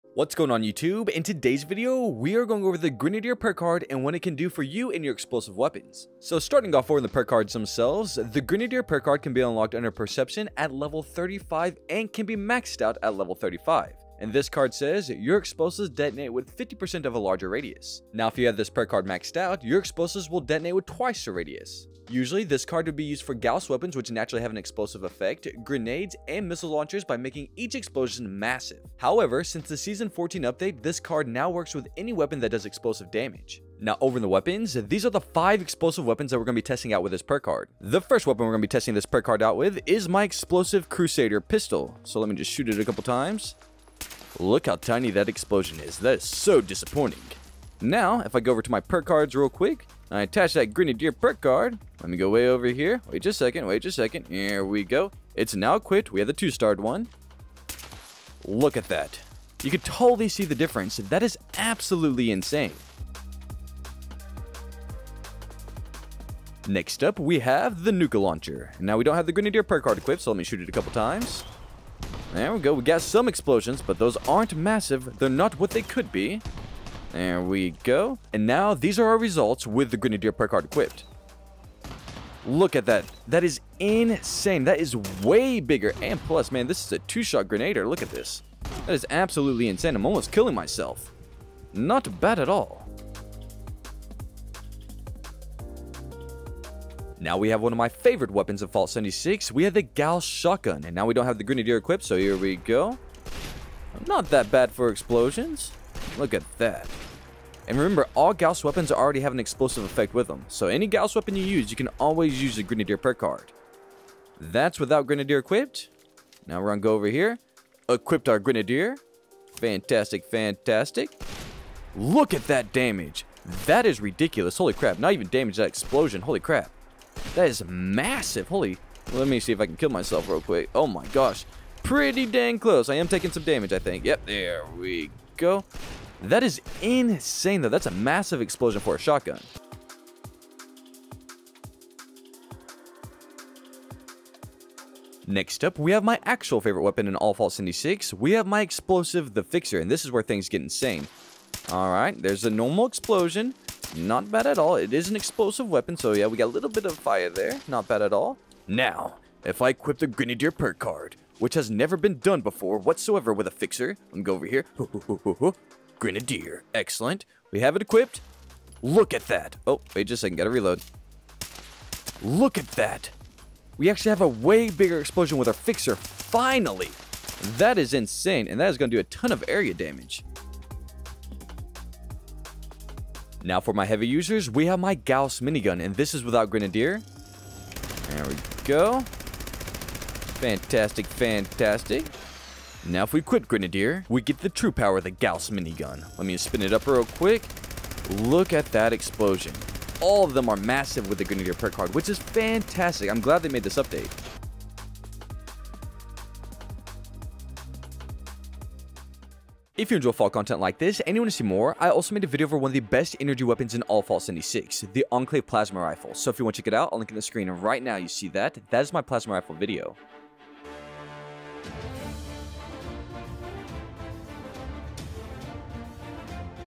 This is a MUST For All Explosive Weapons!!!
What's going on Initiate, Bird here and in this video we are going over the insane perk card grenadier!! Since the season 14 update, this perk card now works with all the explosive weapons and effects in game!! This is one card I can recommend to anyone. I hope this helps you become one of the most powerful vault dweller in the wasteland! AD VICTORIAM!👊🖤